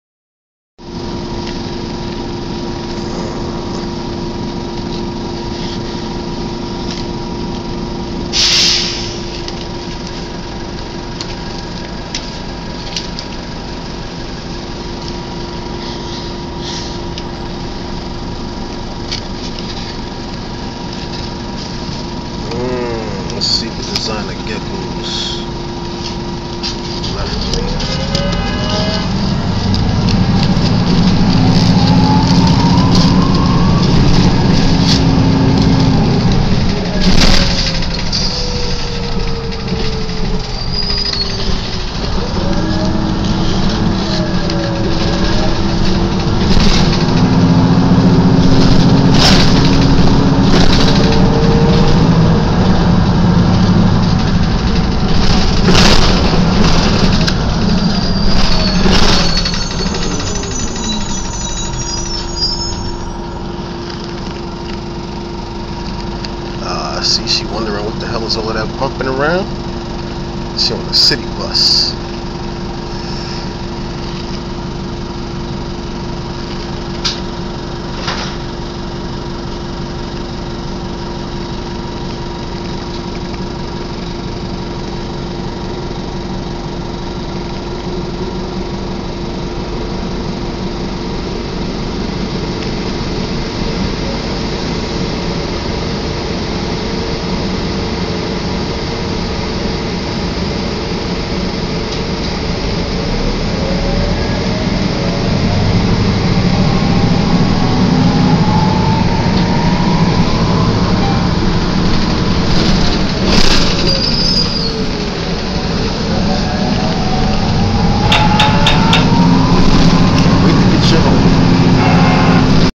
Unboxing the new gecko pt.1 MTA going her way!
Unboxing a gecko I recieved late. Glad she isnt dead. The heat pack was cold stiff when I got to her but she was alright. Time to get her some wax worms when she gets home.